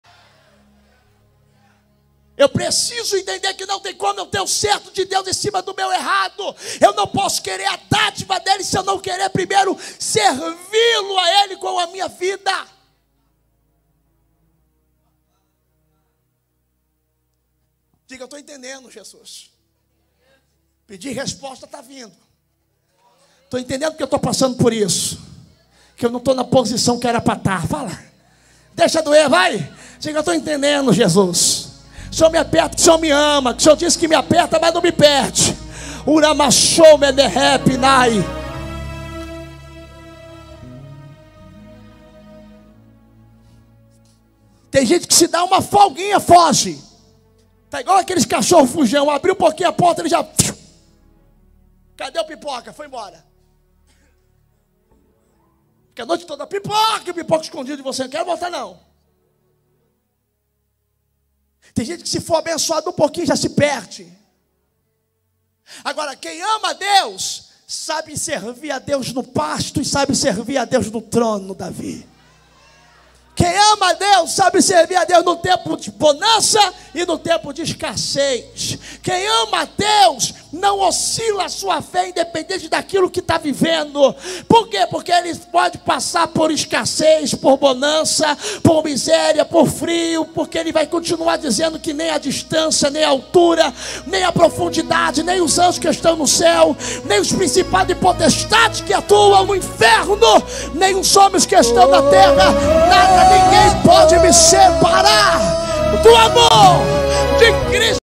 NADA PODE TE SEPARAR DO AMOR DE CRISTO - Pr Leonardo Sale
Forte Mensagem com Pastor Leonardo Sale!

Banco Bradesco:
Ag: 7049 - CC: 15762-7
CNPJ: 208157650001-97